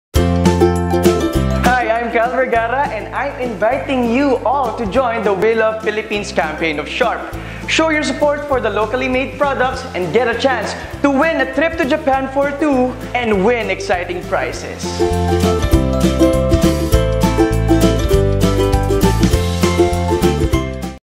Sharp "We Love Philippines" Digital Campaign | Kyle Vergara
"We Love Philippines" Digital Campaign from Sharp Philippines
Featuring Kyle Vergara
October 2018

Shot & Edited by Frame Rivas